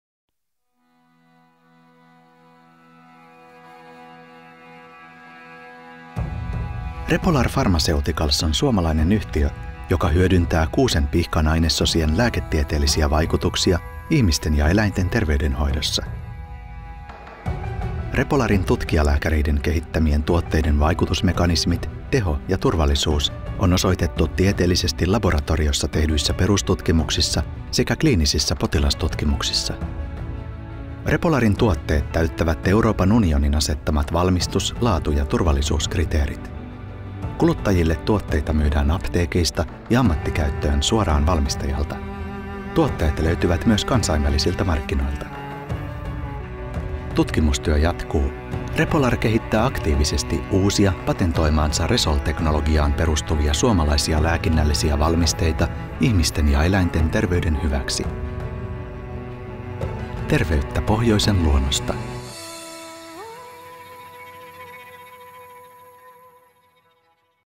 Repolar Pharmaceuticals Oy:n yritysesittely
Repolar Pharmaceuticals on suomalainen startupista hartiavoimin vientiyritykseksi kasvatettu osakeyhtiö, joka kehittää, valmistaa, markkinoi ja myy haavan- ja ihonhoitoon tarkoitettuja valmisteita koti- ja ammattikäyttöön. Tuotteidemme lääketieteellisesti vaikuttavat raaka-aineet ovat peräisin - mistäpä muualtakaan kuin suomalaisesta metsästä. Vihreä kulta ei enää olekaan pelkkää sellua ja pölliä - nykyään se on myös kansainvälisen tason lääketiedettä. Katso yrityksemme tarina ja taustat täältä.